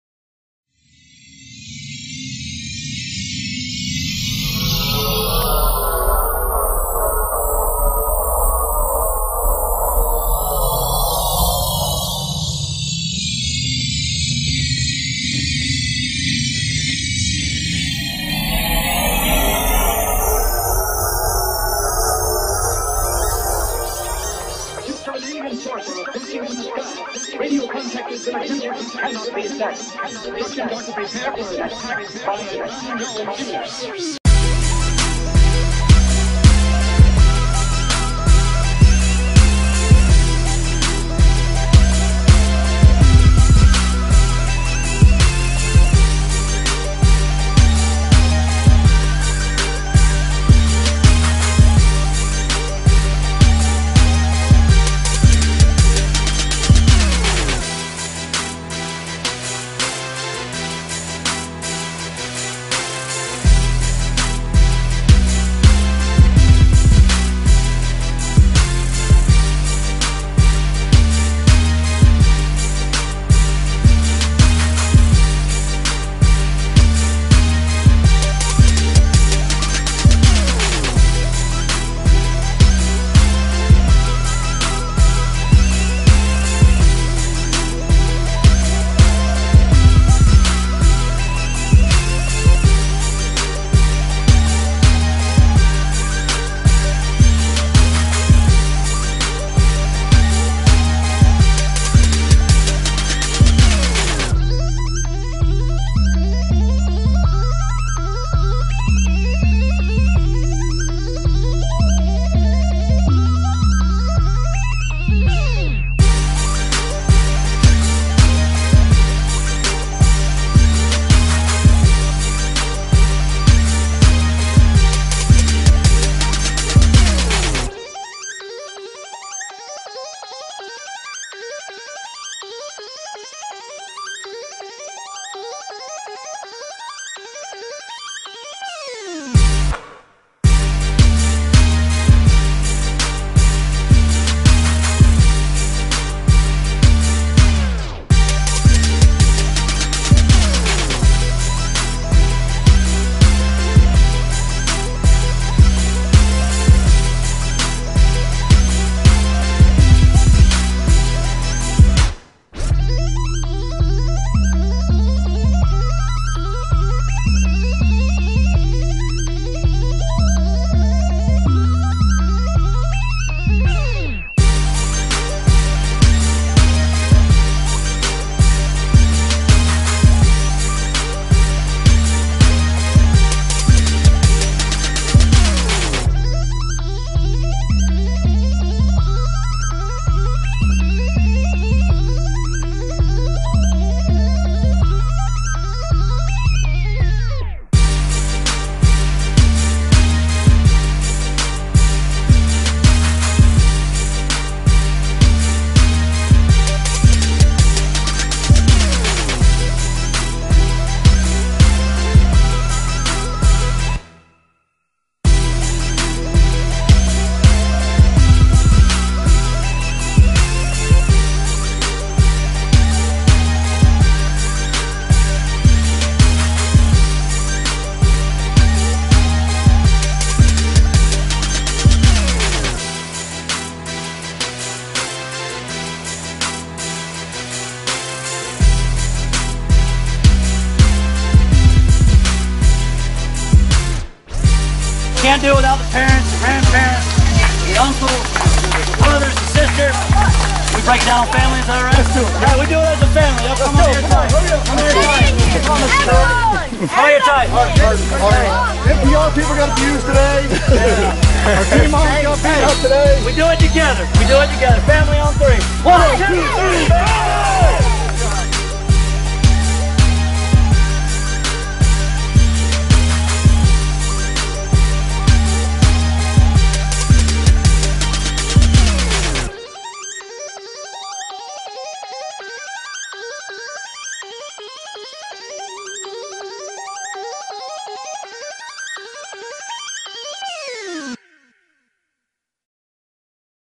Outlaws vs Saints Highlights
2012 Mitey Mite - Outlaws vs. Saints. Outlaws WIN